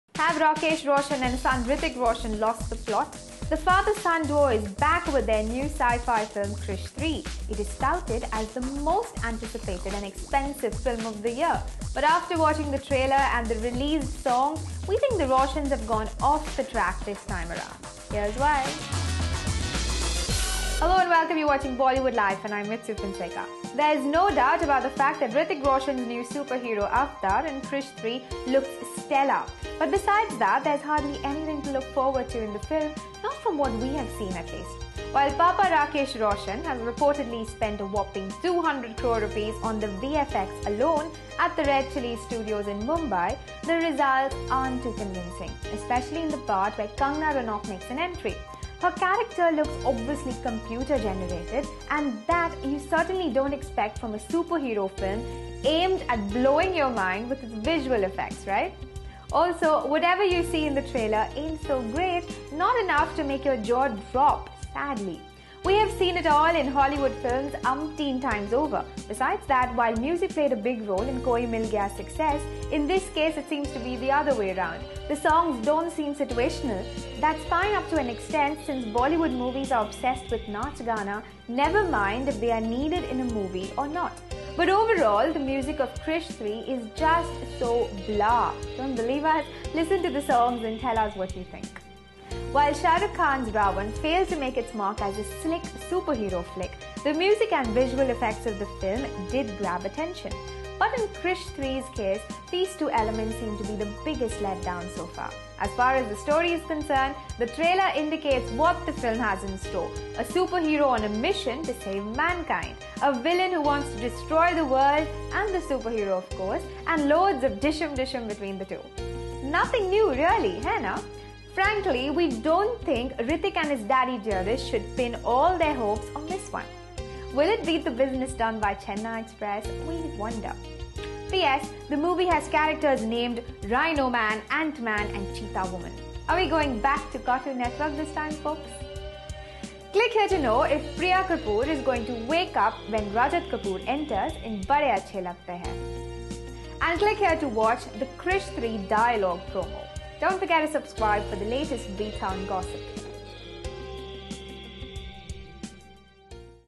Have Rakesh Roshan and son Hrithik Roshan lost the plot
The father-son duo is back with their new sci-fi film Krrish 3. It is touted as the most anticipated -- and expensive -- film of the year. But after watching the trailer and the released songs, wethinks the Roshans have gone off track this time around. Here's why